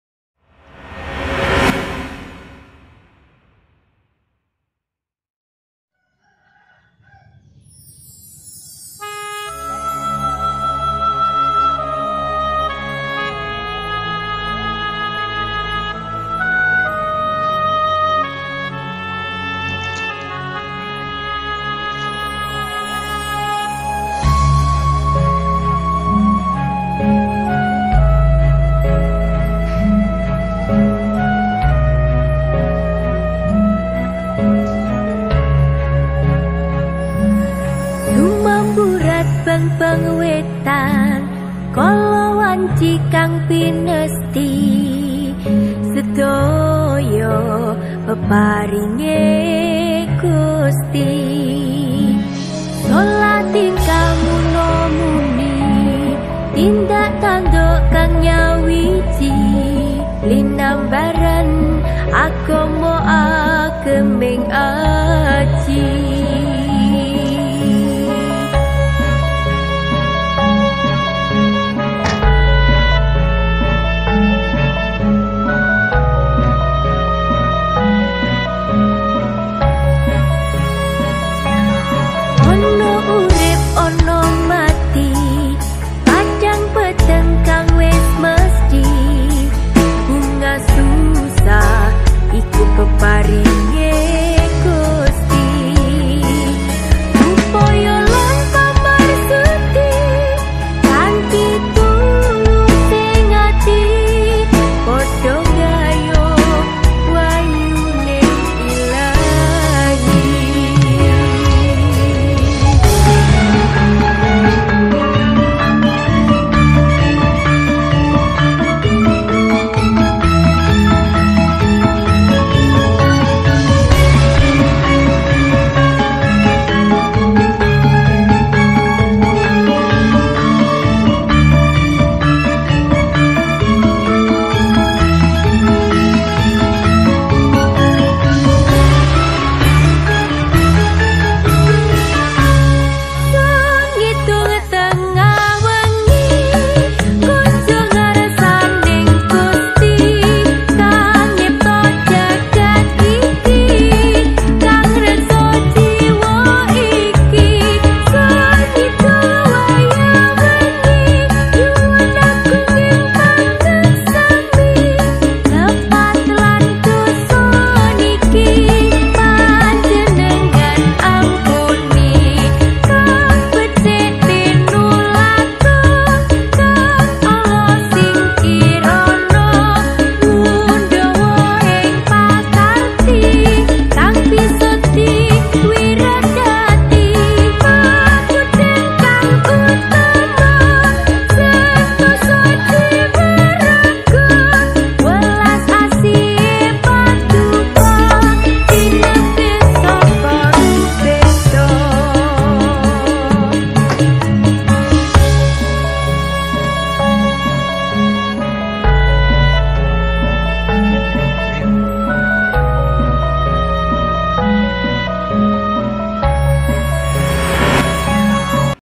MIN 1 GUNUNGKIDUL SALURKAN BANTUAN BHAKTI SOSIAL UPZ KEMENAG GUNUNGKIDUL
Video berikut merupakan dokumentasi pelaksanaan kegiatan MIN 1 Gunungkidul salurkan bantuan Bhakti Sosial UPZ Kemenag Gunungkidul dalam musibah Covid 19 Tahun 2020

Sasaran penyaluran,  Siswa dan siswi yang terdampak, masyarakat sekitar lingkungan madrasah yang memerlukan.

Bantuan disalurkan langsung olek kepala min 1 Gunungkidul, didampingi 2 guru.

Musik by Ardia Diewangi Probowati